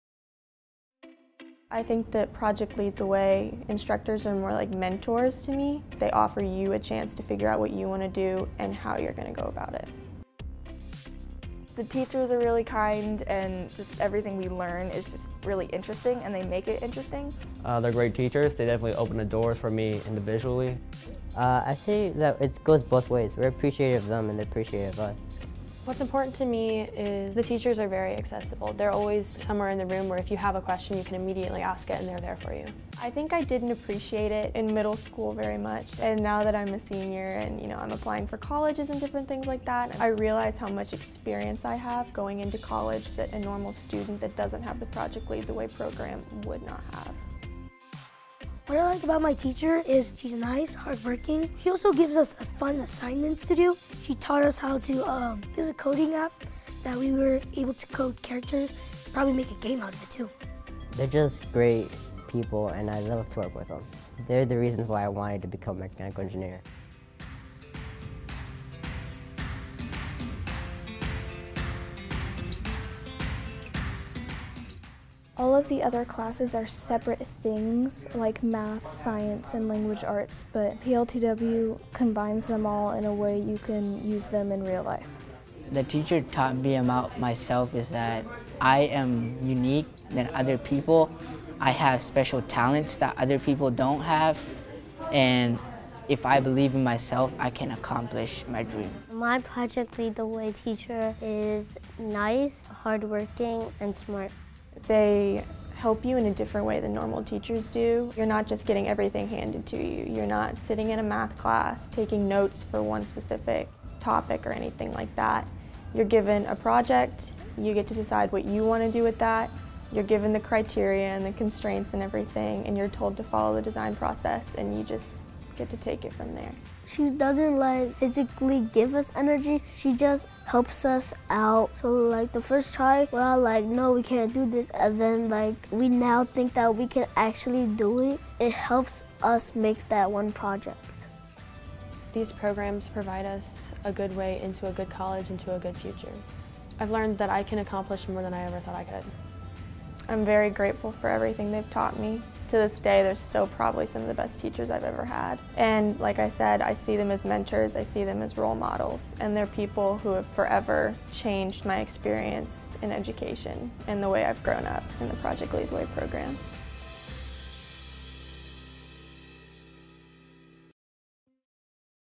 Unlocking the Potential of PLTW Teachers and Students
Students can't be what they can't see, and PLTW amplifies the potential in all students by providing them access to hands-on education that prepares them to thrive in our rapidly evolving world. In this video, look inside the classroom and see the real-world impact of PLTW.